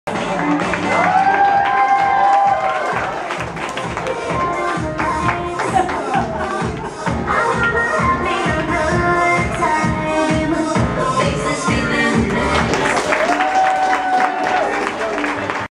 Joshua Modeling for John Casablancas
Joshua had a "great time" as did everyone in the audience  when he was on the runway at John Casablancas Model and Talent Career Centers, Directed by Greg Finenco.   9 years old, Modeling, Runway, John Casablancas